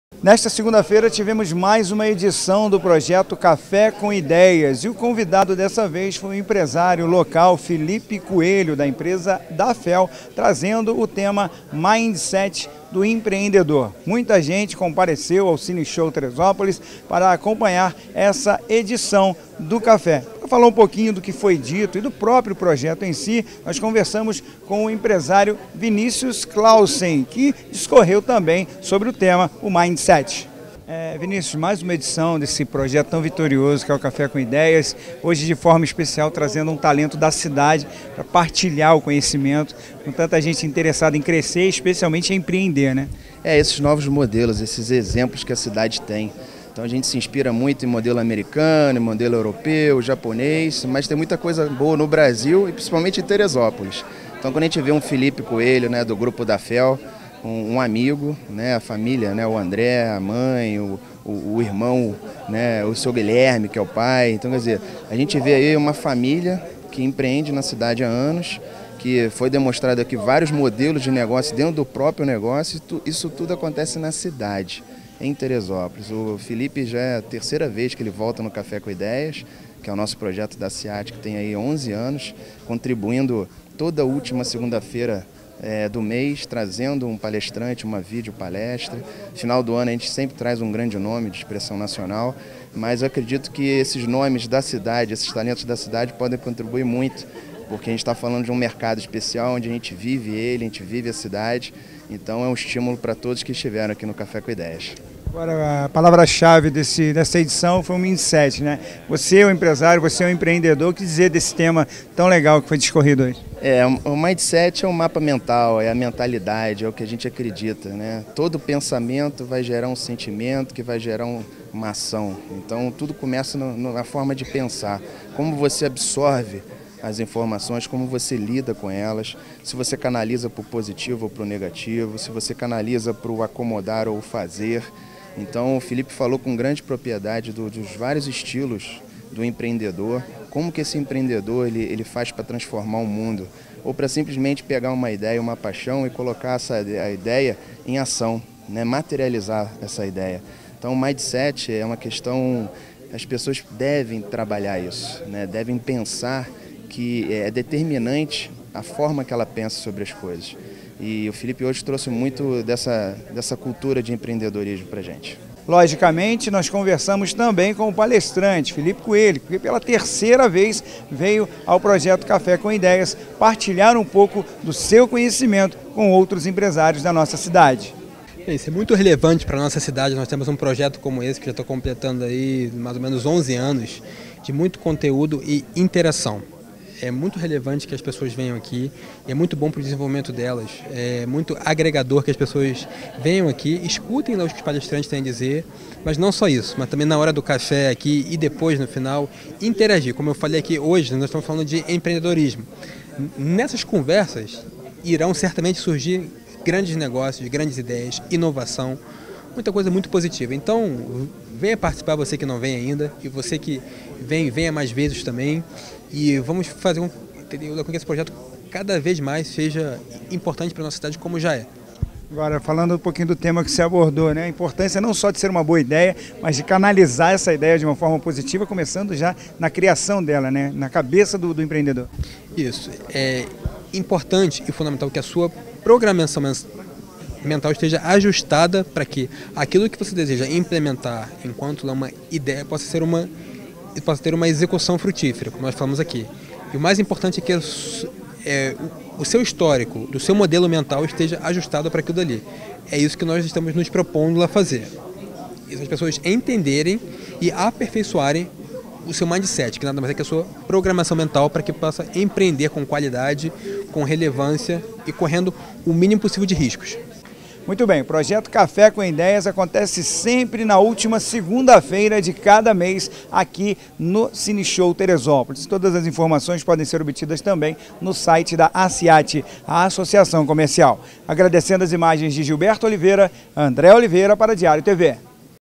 Casa cheia para Felipe Coelho
Empreendedorismo é o tema de mais uma edição do projeto